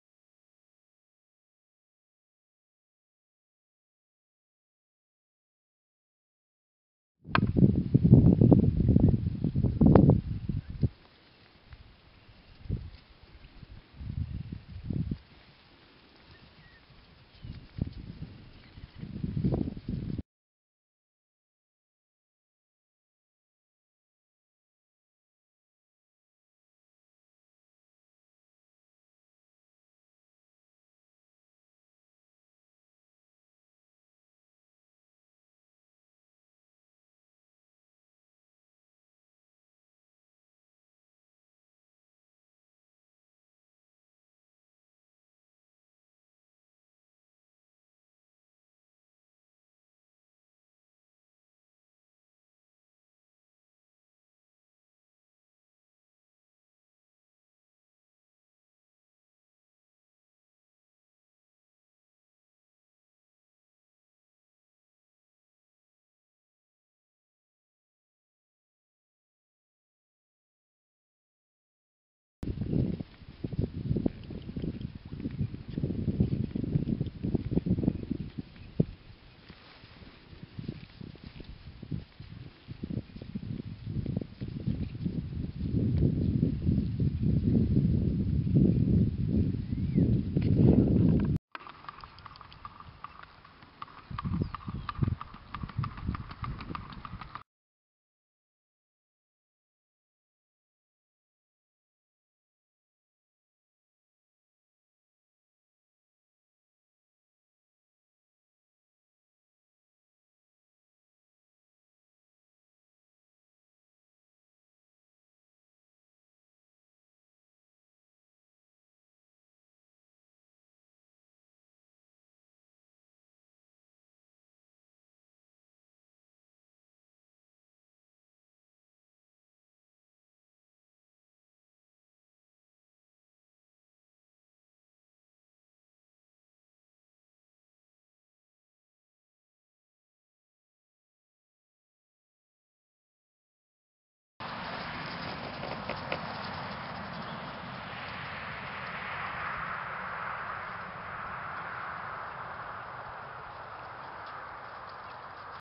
Gaited Morgan Gelding Missouris Pretty boy 2013
By Lovely katie gaited morgan mare and Missouris Dandy Boy gaited Morgan stallion